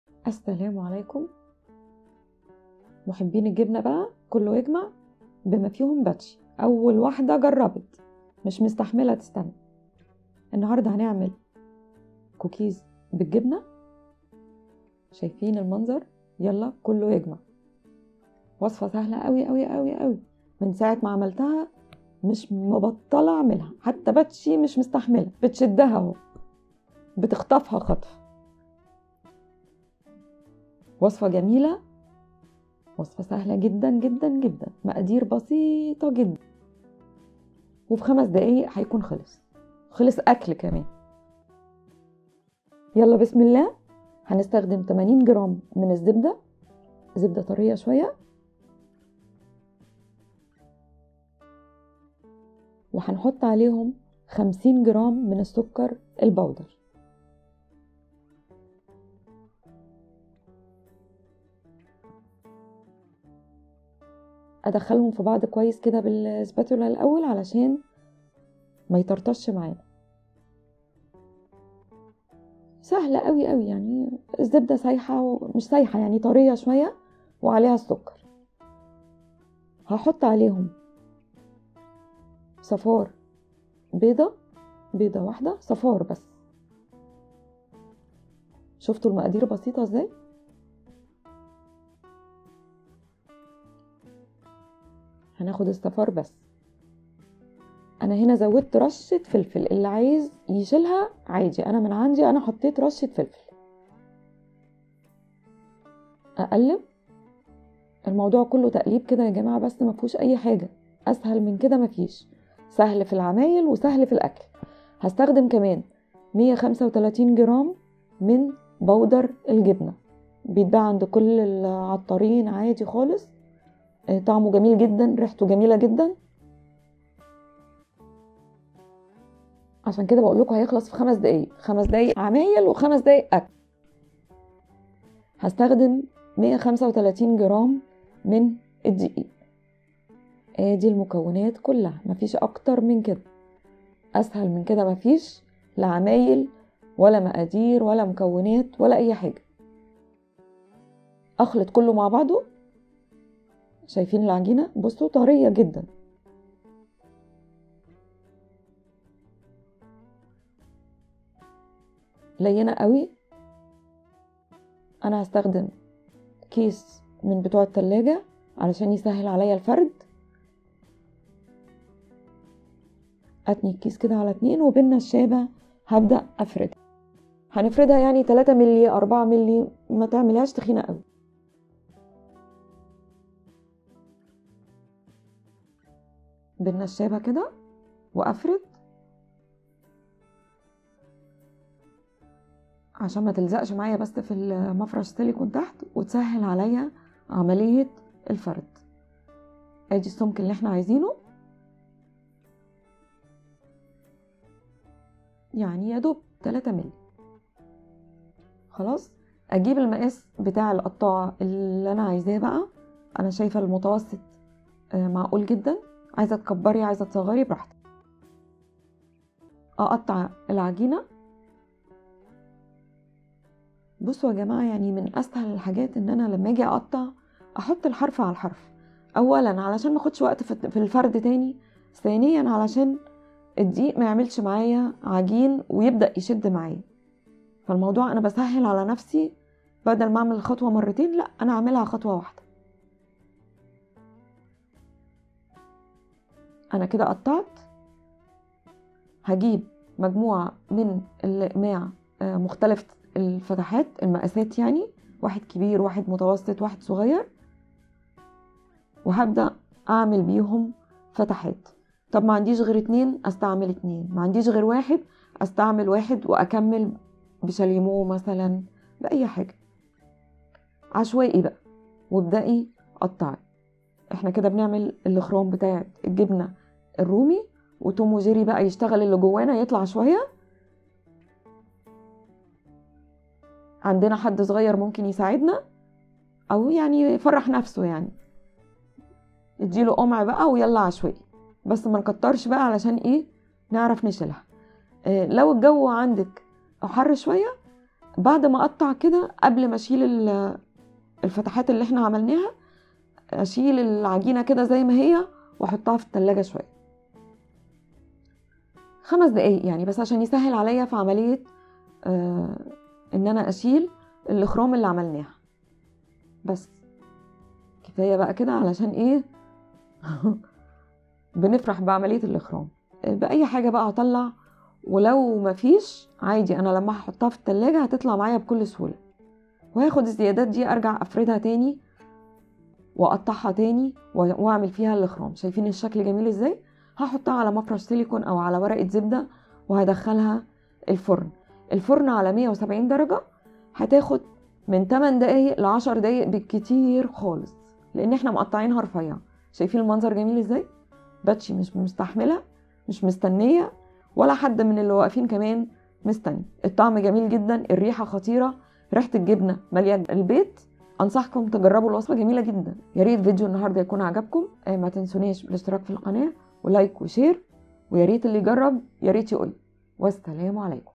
بسكوت الجبنة , ب طريقه تحضير سهله و سريعة وصفة لذيذة جدا
مقادير او مكونات وصفة بسكوت الجبنة اللذيذ :
80 جرام زبده لينه
50 جرام سكر بودره
صفار بيضه
رشه فلفل اسود ( اختياري)
35 جرام بودره جبنه
135 جرام دقيق

درجه حراره الفرن 170 درجه
من 8 -10 دقائق
يمكنك التواصل معنا عبر الفيس بوك